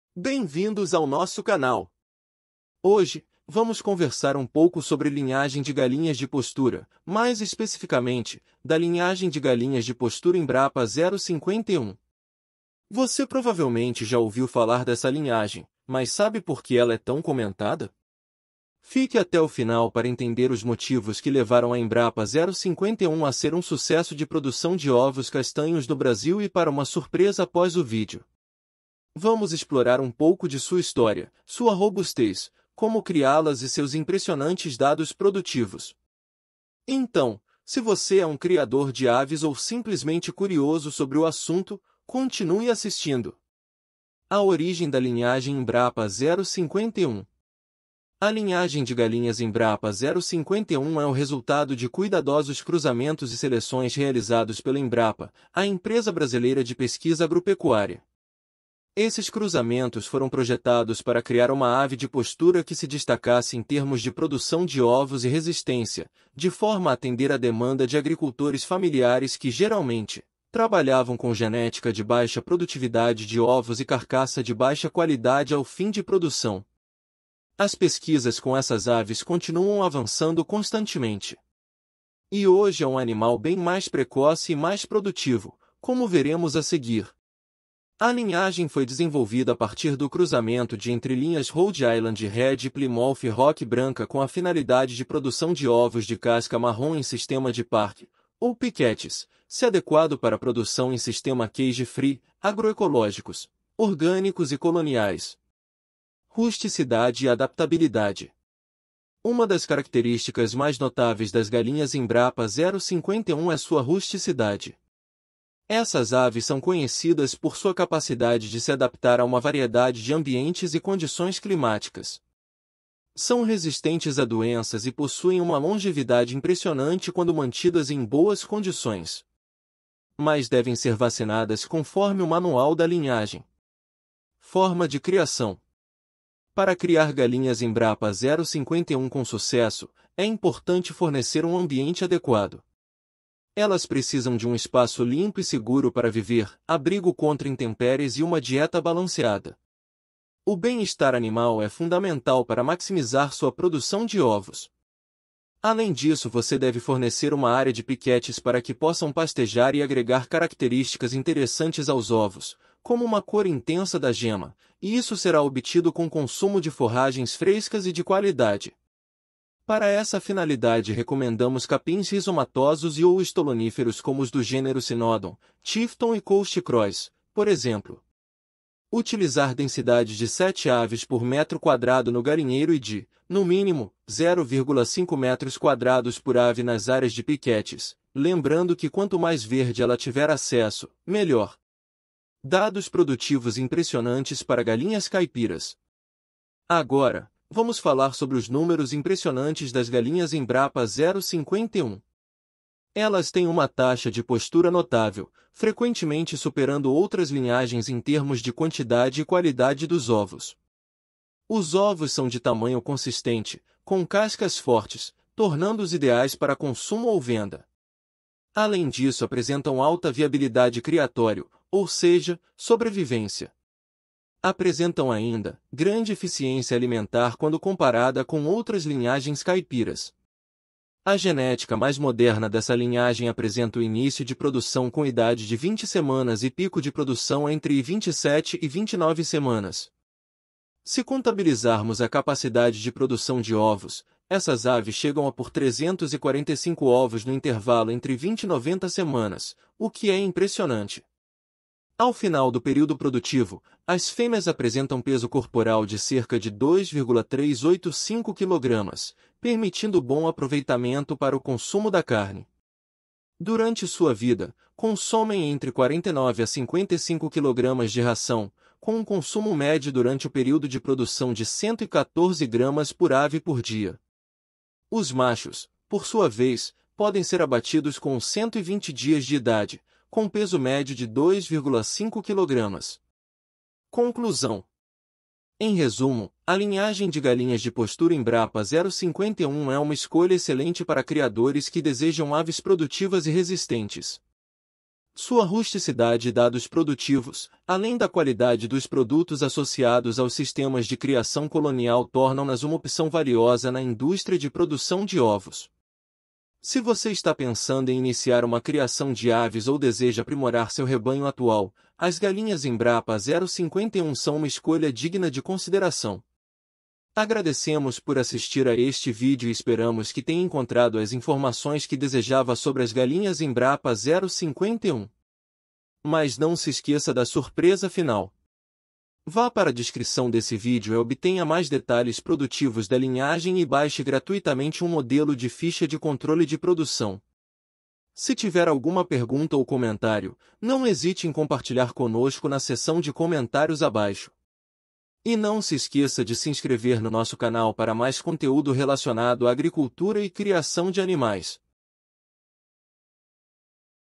Galinha de postura EMBRAPA 051
Produção de ovos é com ela mesmo, alinhando rusticidade, eficiência alimentar, docilidade. Essa galinha pode produzir mais de 300 ovos.
crédito das imagens: Tv Embrapa
Link para baixar modele de controle e conhecer mais detalhes técnicos da EMBRAPA 051.